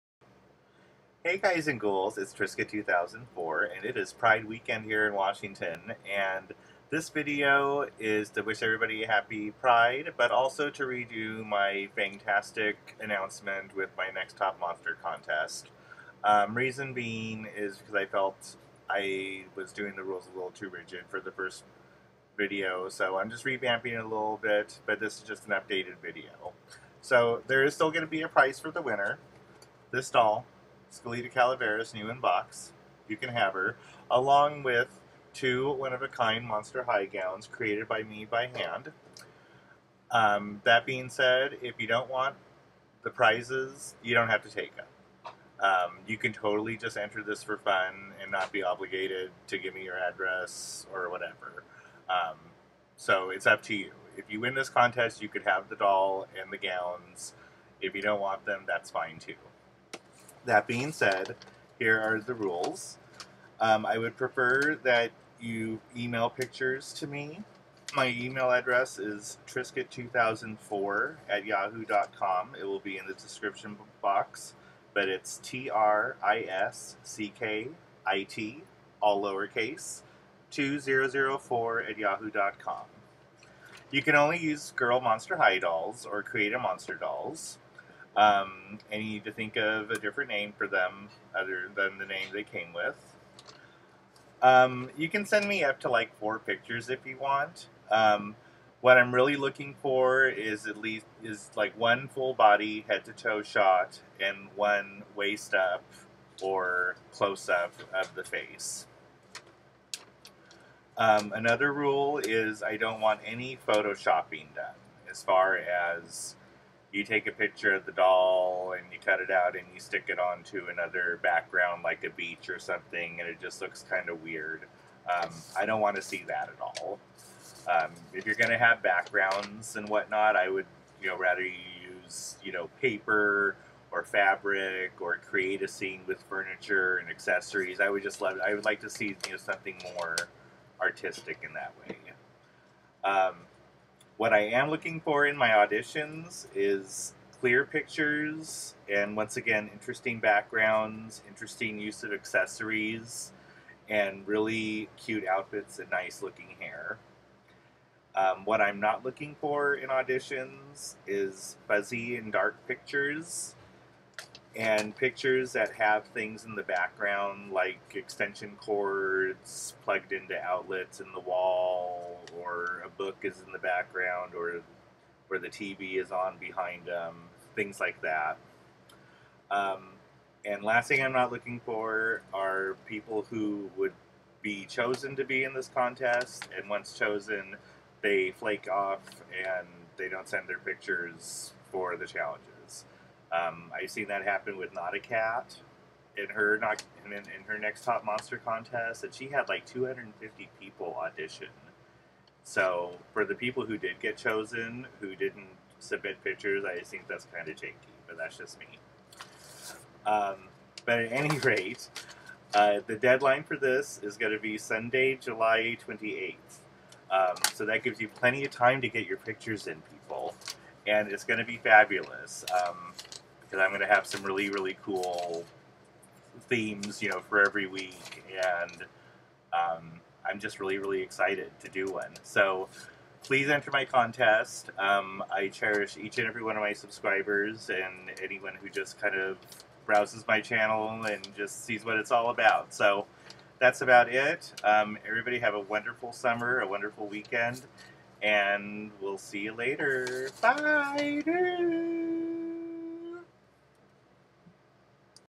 Fangtastic NTM Contest : UPDATED VIDEO
Hey Guys and Ghouls,

Update UPDATE: To avoid confusion I am deleting the original video

I am so excited to announce open auditions for my upcoming inaugural season of Next top monster. I am doing this not only because it is so fun but as a thank you to all my clawsome subscribers.

OFFICAL RULES:

1. Grand Prize is one NIB "skeleta Calaveras" doll and two (2), one of a kind hand made monster high dresses. You don't have to take the prizes if you don't want them or your parents don't want you giving your address out. You will have the option of me mailing you the prize or if not, bragging rights to winning and I'll probably create some glittery, fabulous tribute video for you

2. You can only use girl Monster High dolls and please think of a name for her ( i may be willing to make an exception to the name rule per my discretion)

4. For this audition, you may send me up to four pictures. My only request is that one is a head to toe body shot and one is either a torso or close up shot. I do not care what background or theme you decide on.

5. Regarding photo editing, the only thing i do not want to see at all in this competition is photoshopping of your model onto a background. No offense to anybody, but it is not my thing and I prefer photos to actually be staged.

6. Lastly, I ask that if you are serious about entering and in the event you are chosen, to please, submit a photo for every challenge you make it to and not just flake off an opportunity someone else would have wanted. NotaCat had over 250 auditions for her last NTM and the fact that people were chosen and just didn't bother to participate is pretty messed up IMO

Things I am looking for in auditions:
-clear, bright pictures
- good use of a background
- good use of props and/or accesories
-cute outfits and hair styles

Things I am not looking for in auditions:
- dark, blurry, and/or grainy pictures
-backgrounds that you can tell is staged ( example: you use red paper, and in the corner, there's the kitchen floor)
- random things in the background (example: extension cord plugged into the wall, a light switch, the Tv is visible and on in the background, random household items,)

The, no exceptions deadline is Sunday, July 28th. Premiere video will be uploaded by the 30th, announcing the contestants and the first challenge.

That's about it. I hope to get lots of inquiries as I am super excited to do this for all of you. Please do not hesitate to ask any questions

Good Luck!!!